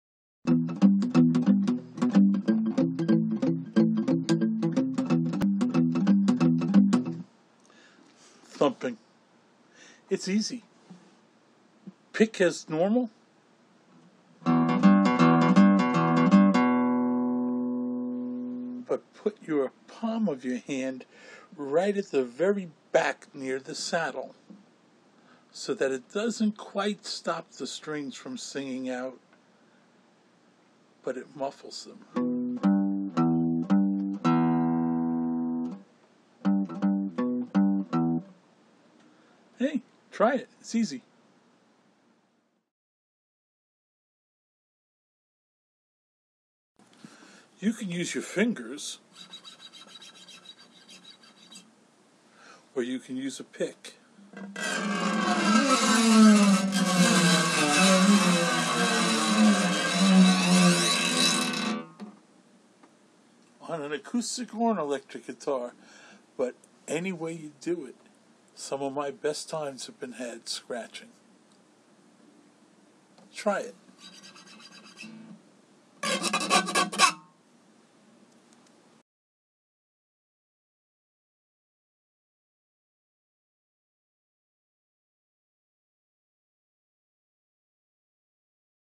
Thumping and Scratching Two Techniques in a Minute and a Half - Theory Shorts, The Bottom Line
Quick to learn but best used sparingly. These two techniques will quickly grab attention.
Remember tact and when you want to make a statement these 2 techniques will help - Jopdu